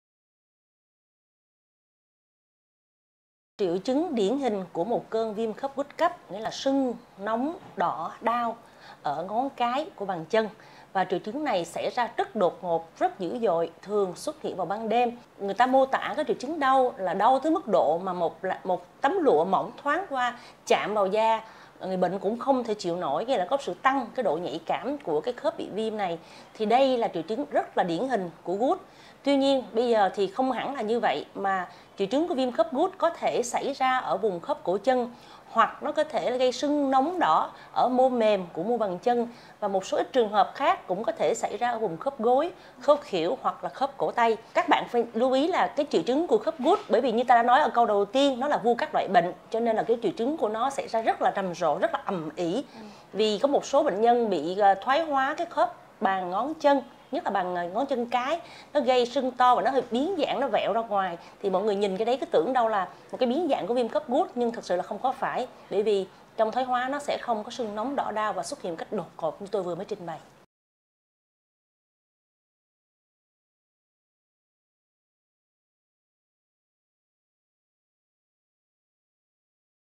Triệu Chứng Viêm Khớp Gout Cấp| BS CK2 Đoàn Thị Huyền Trân
Bệnh gout là một dạng của viêm khớp, gây đau, sưng và nóng ở khớp. Bệnh gout được coi là một dạng viêm khớp phổ biến và rất phức tạp. Các khớp chân, ngón chân, mắt cá chân và đầu gối là những vị trí phổ biến nhất và bị ảnh hưởng nhiều nhất bởi gút.